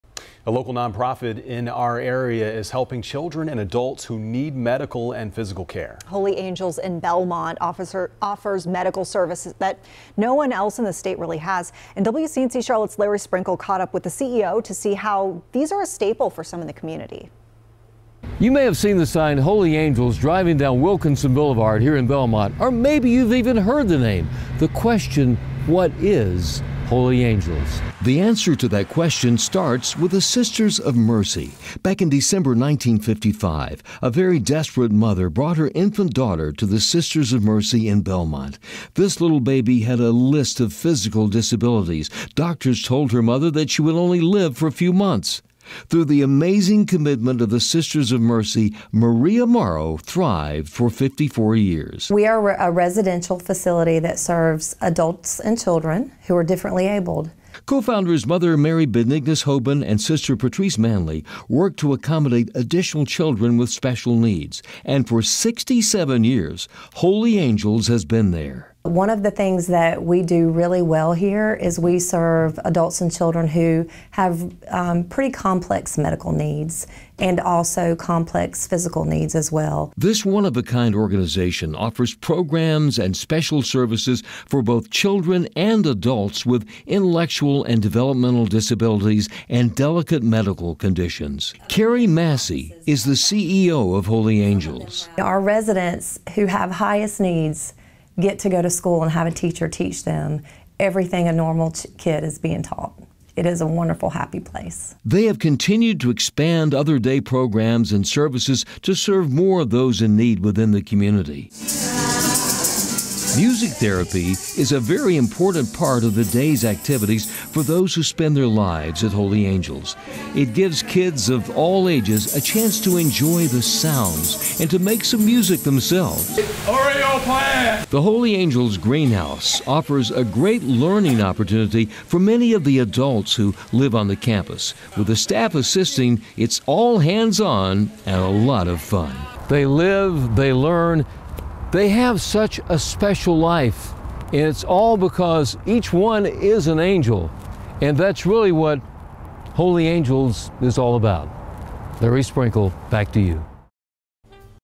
Holy Angels providing needed care for Belmont community
It’s a well-known landmark in Belmont, North Carolina on Wilkinson Boulevard -- the Holy Angels sign. You may have seen the sign but you’re not sure about the name Holy Angels. It all starts with the Sisters of Mercy.

The dedicated group has always been at the forefront when it comes to the needs of others.

Everything started back in December 1955, when a very desperate mother brought her tiny infant daughter to the Sisters. This little baby had severe disabilities, including spina bifida and hydrocephalus. Doctors actually told this young mother that there was no way the baby would live beyond a few months. Through the amazing commitment and dedication of the Sisters of Mercy, Maria Morrow lived for 54 years.

Co-founders Mother Mary Benignus Hoban and Sister Marie Patrice Manley worked to accommodate the additional children with special needs who began to arrive at the newly formed Holy Angels.